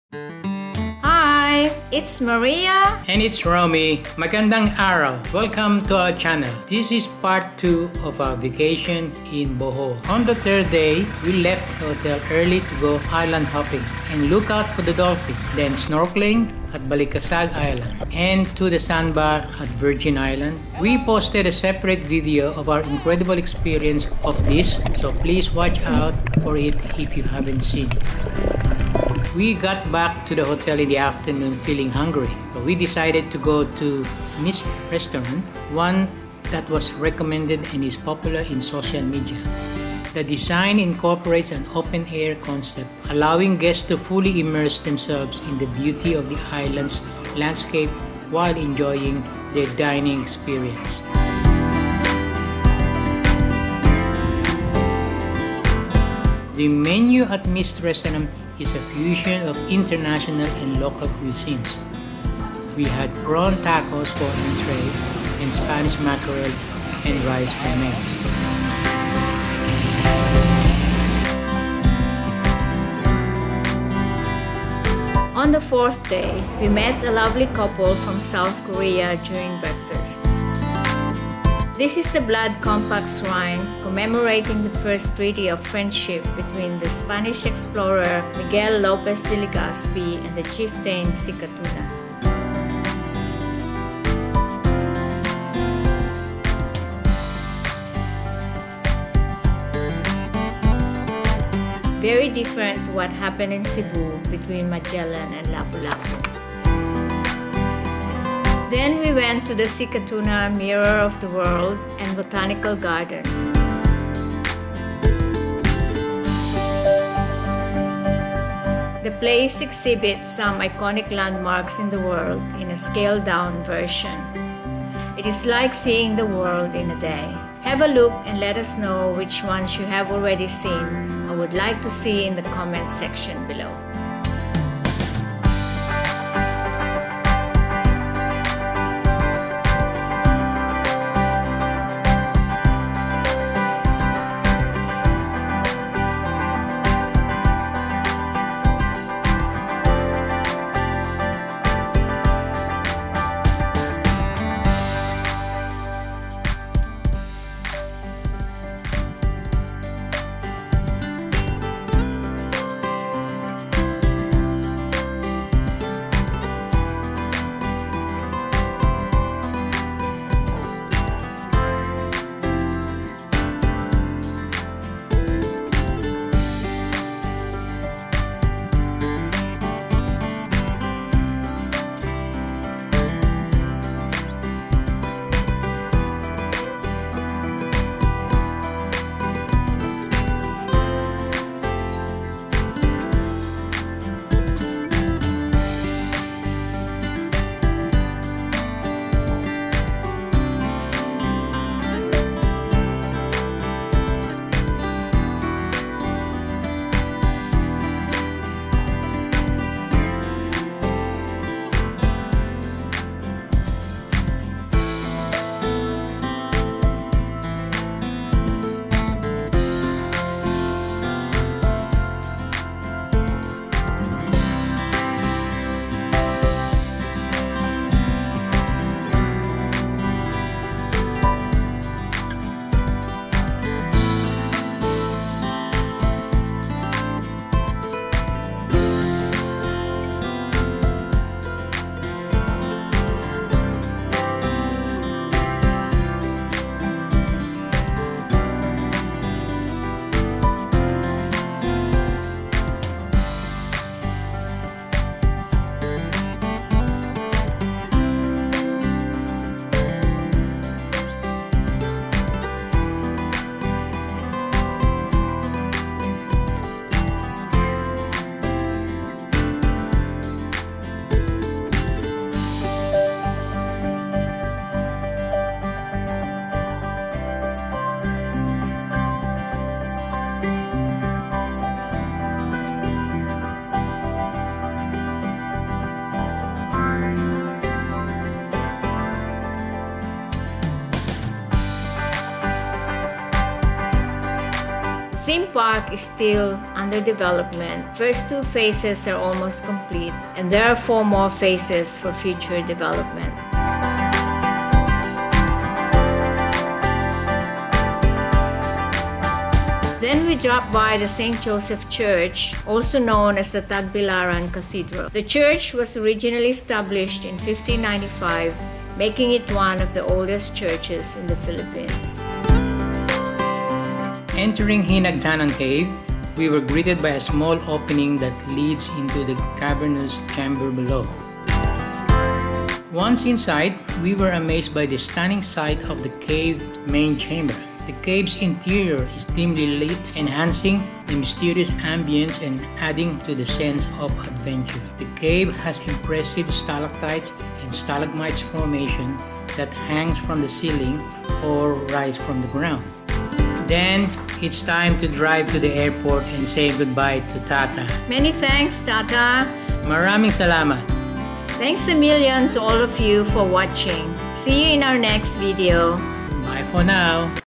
Philippines Travel - Bohol Itinerary Part 2 - Mirror of the World and more
This is part 2 of our itinerary when we visited Bohol, Philippines on May 2023. Join us as we also show you the sights of the world in beautiful Bohol, Philippines.

00:00 Introduction
00:10 Island Hopping
00:30 Mist Restaurant
01:20 The Blood Compact Shrine
01:49 The Mirror of the World and Botanical Garden
05:10 Tagbilaran Cathedral
05:23 Hinagdanan Cave